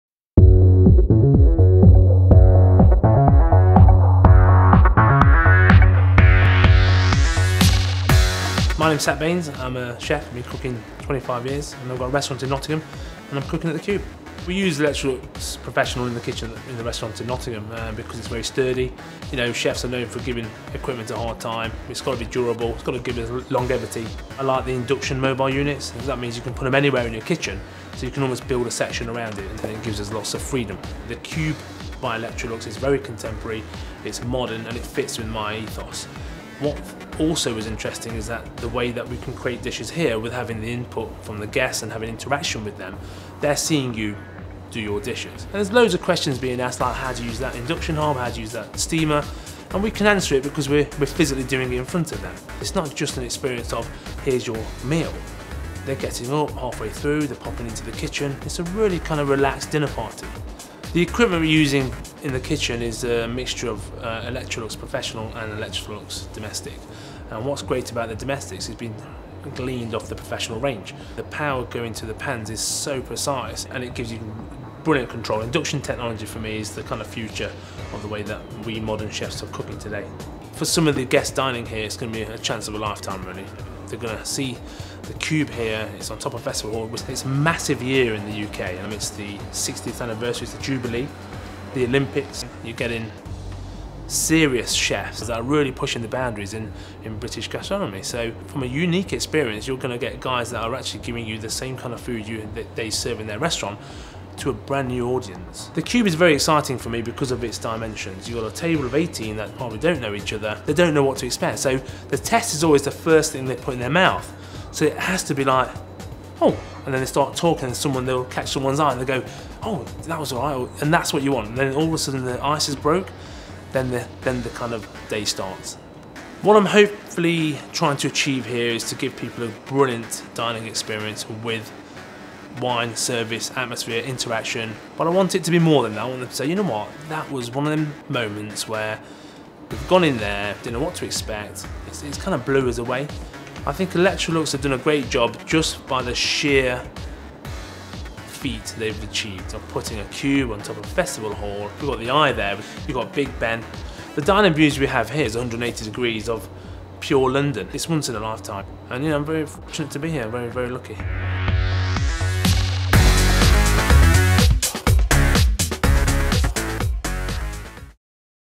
Chef Sat Bains at The Cube London
Double Michelin star chef Sat Bains is the chef-in-residence at The Cube by Electrolux.

The Cube by Electrolux is a touring gastronomic concept that brings to life the unique relationship Electrolux have with some of the world's best chefs.